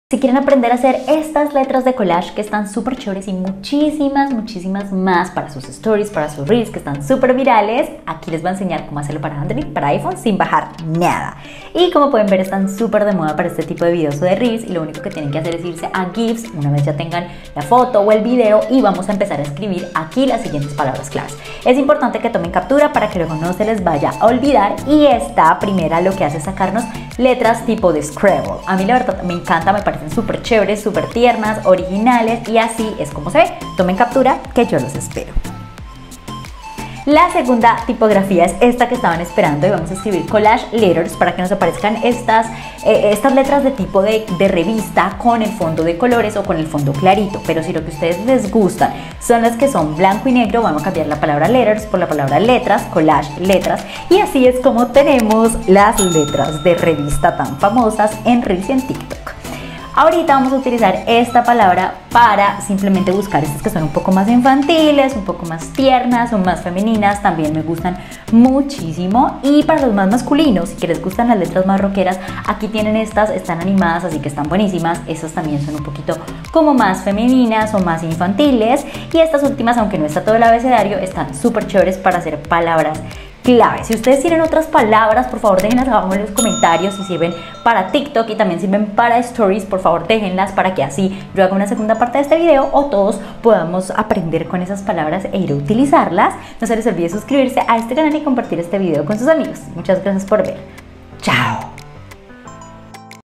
LETRAS DE COLLAGE DE REVISTA PARA STORIES Y TIK TOK 💖🔥
💖🔥QUIERES HACER ESTAS LETRAS DE RECORTE DE REVISTA EN TUS REELS O EN TUS STORIES O TIK TOK, TE EXPLICO CÓMO. 💖🔥